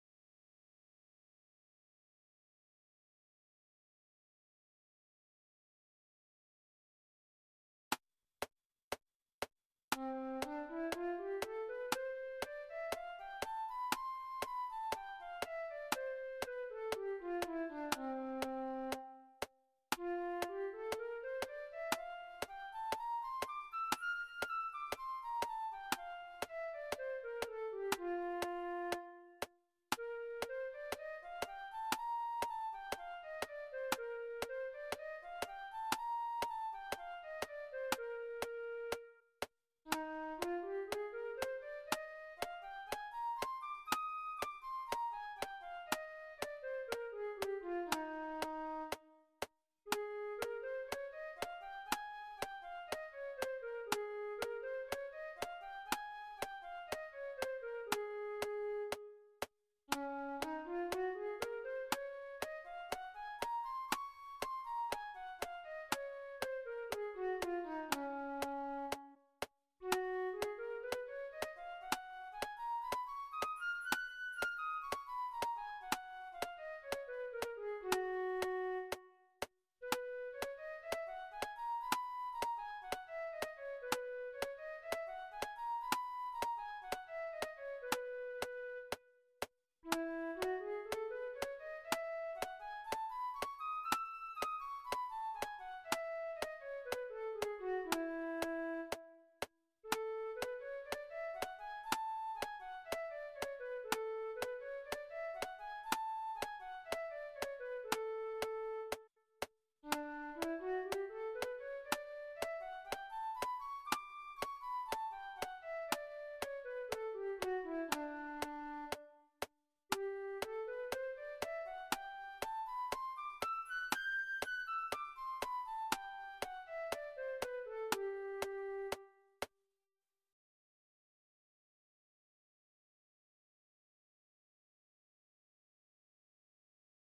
Flute: Multiple Octave Major Scales with Metronome
All 12 major scales in multiple octaves within a range that a medium/advanced flute player should be able to achieve. This should be practiced individually by the student but may be practiced with other instruments as a full band. This solo track has a metronome track as well. For a non-metronome track, see the 'full band accompaniment video.